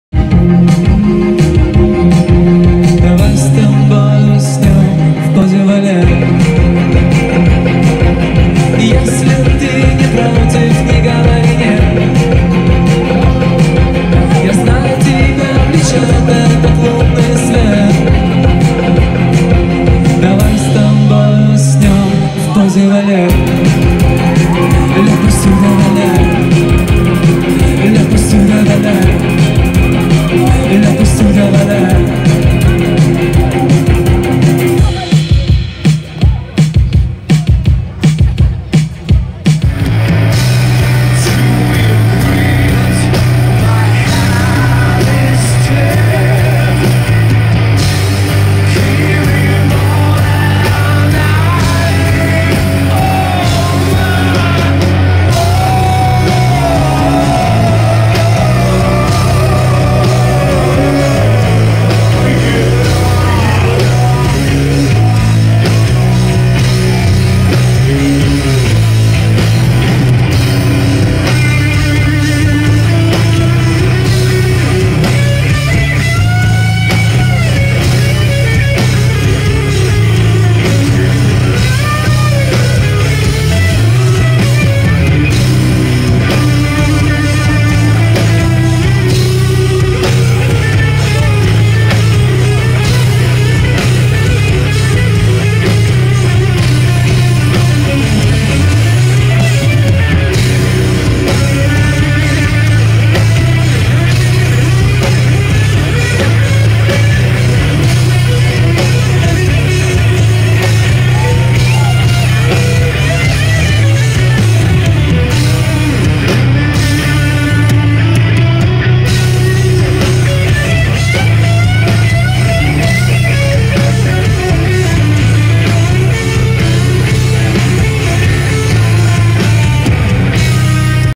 Рок-концерт в Дружковке: УрбанФест-2018
Заключительным аккордом Дружковского УрбанФеста-2018 стало выступление трёх киевских рок-групп.